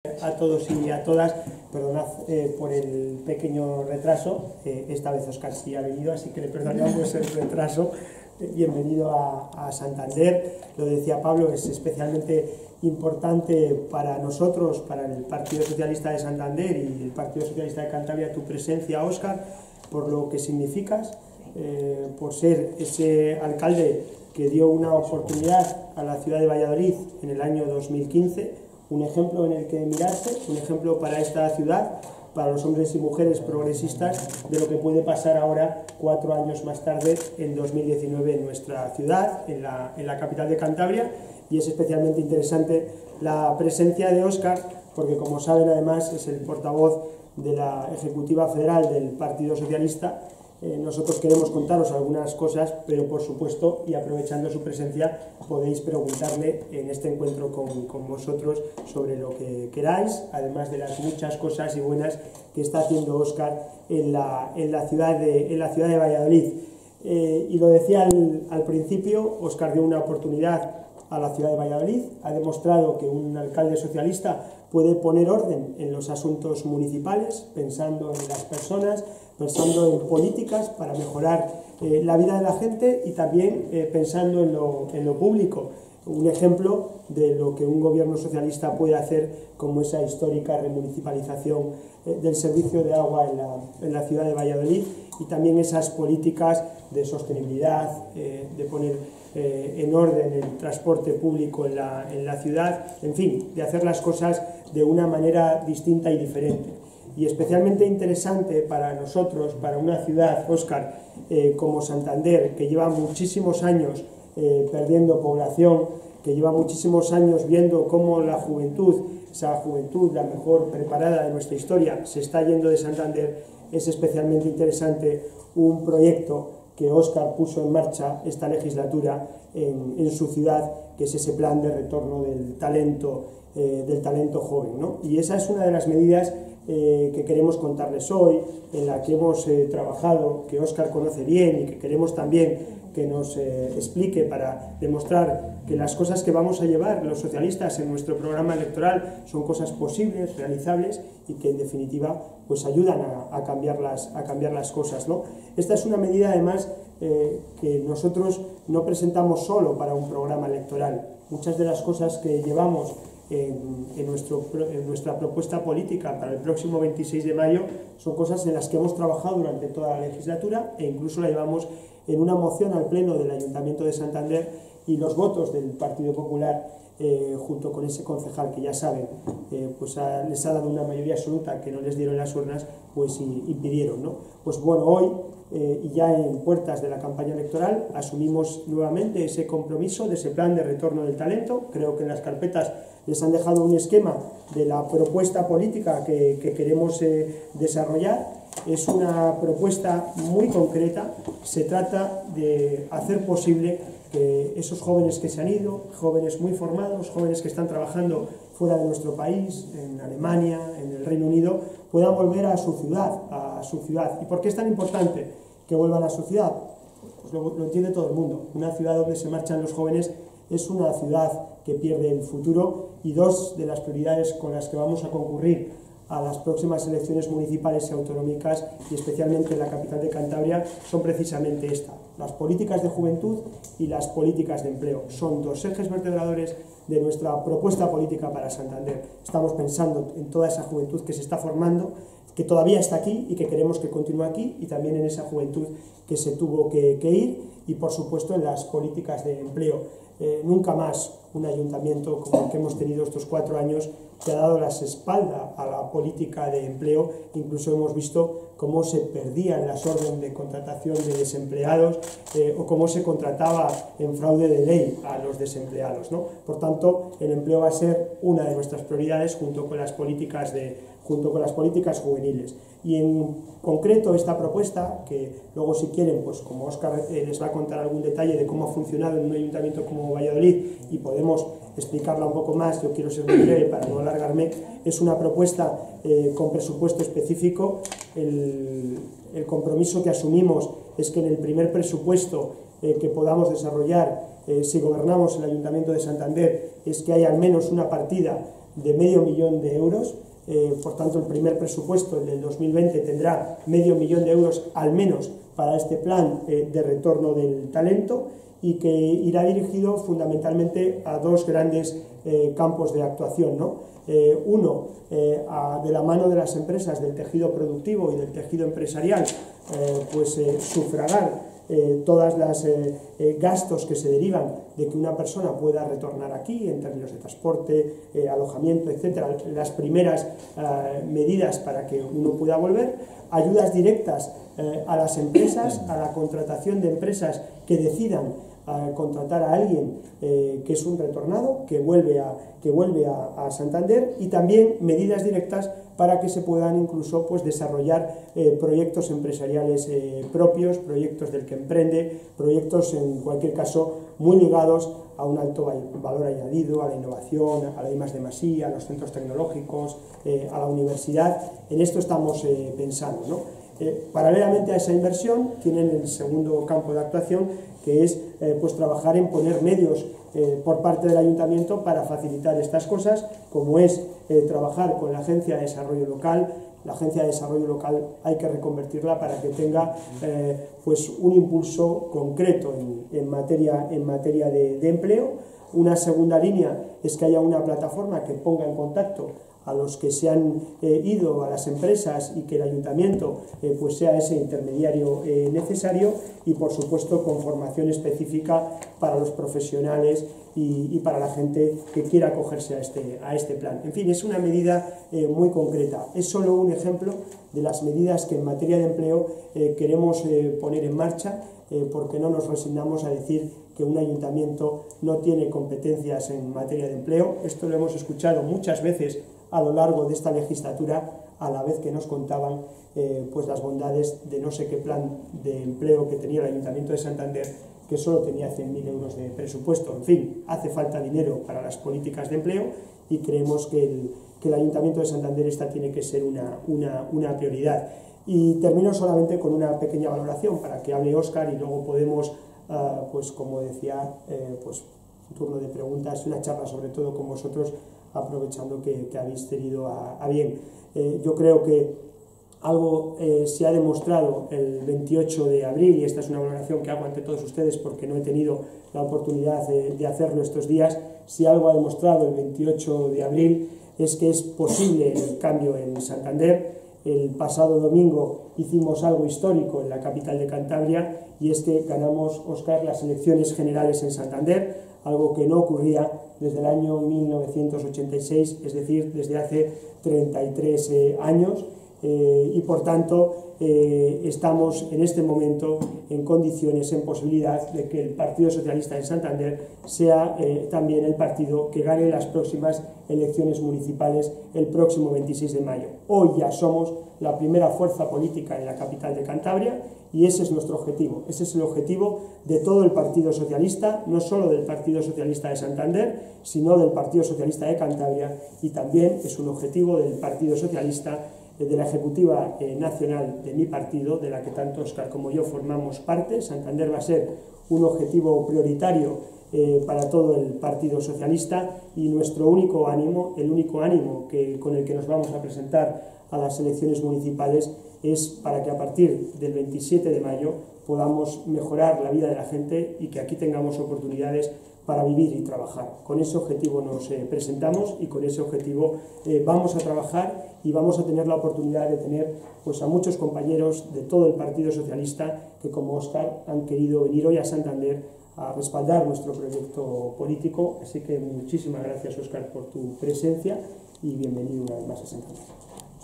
Óscar Puente, alcalde de Valladolid, respalda a Pedro Casares en su proyecto de retorno juvenil
Puente, que ha sido preguntado por los medios durante un acto de partido en Santander, ha aludido también en su respuesta a la "coherencia" del secretario general del PSOE y presidente en funciones, Pedro Sánchez, que optó por "separar" los dos procesos electorales precisamente para respetar los debates en torno a ellos, esto es, que se hable de cuestiones nacionales o autonómicas, municipales y europeas.